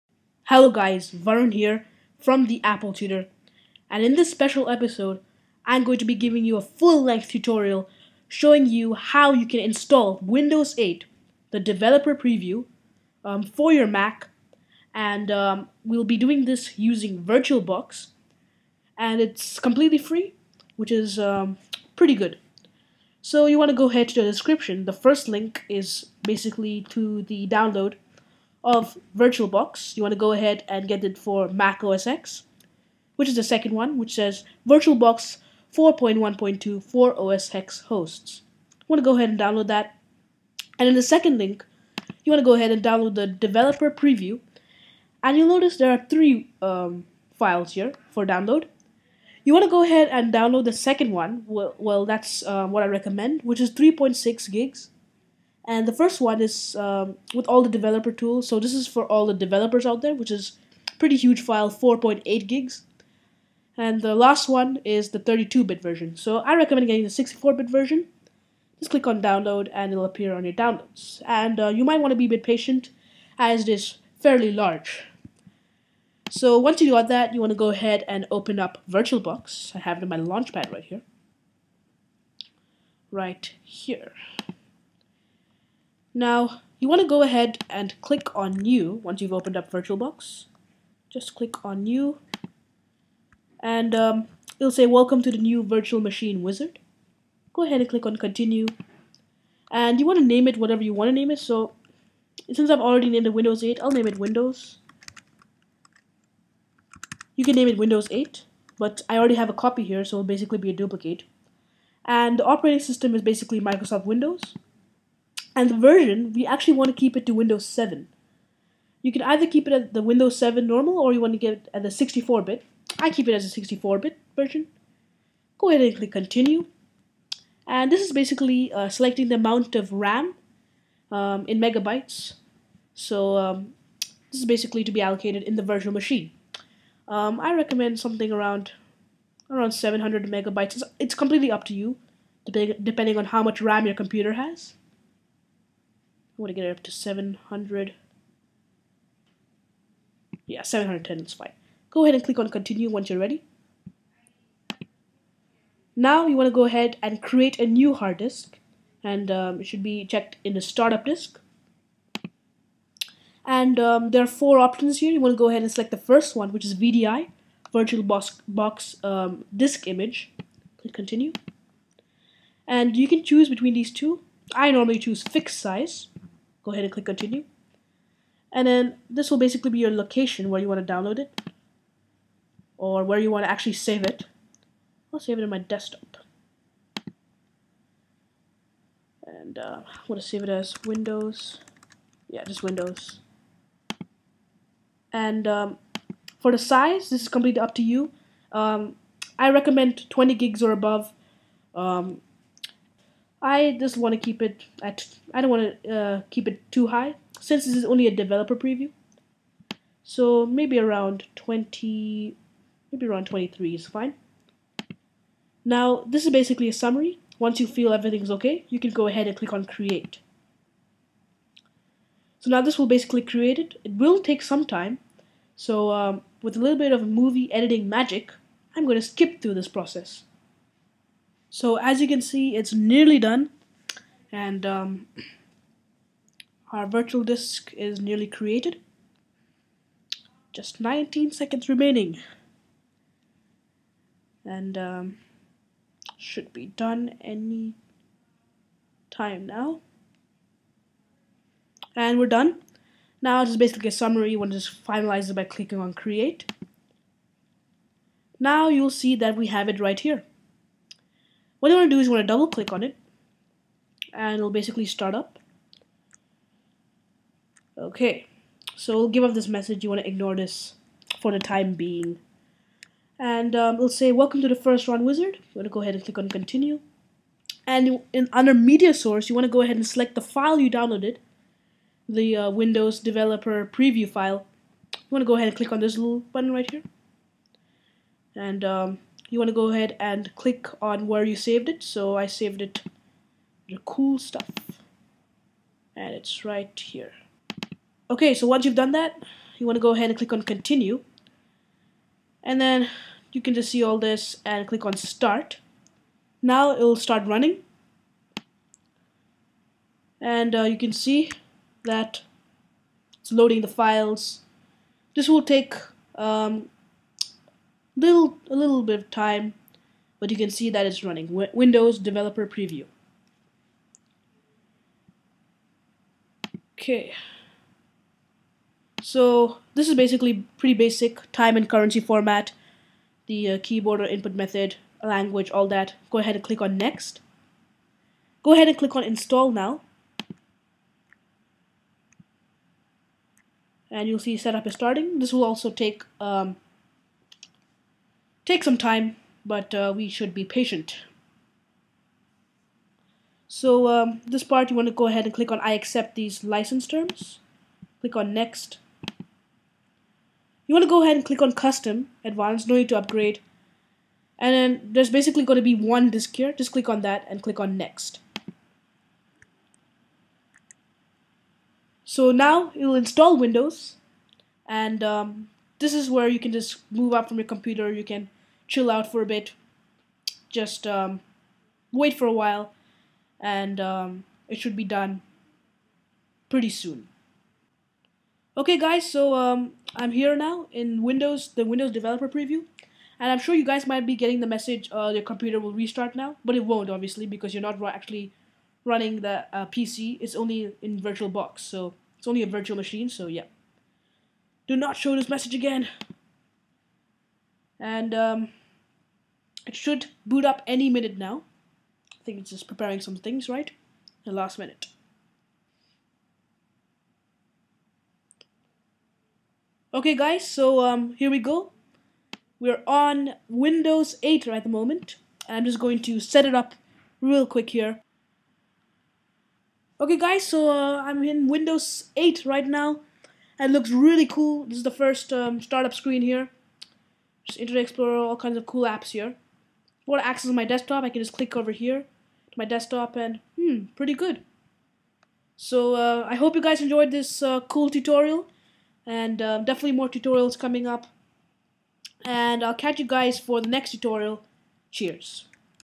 How to Install Windows 8 on Mac using VirtualBox
A detailed and full-length tutorial showing you how to install Windows 8 on your Mac with the help of VirtualBox. If you want to change the screen resolution, simply right-click on the desktop, and select screen resolution.

The iso images can be downloaded at the bottom of the website, where you can choose from the 32 bit or 64 bit versions.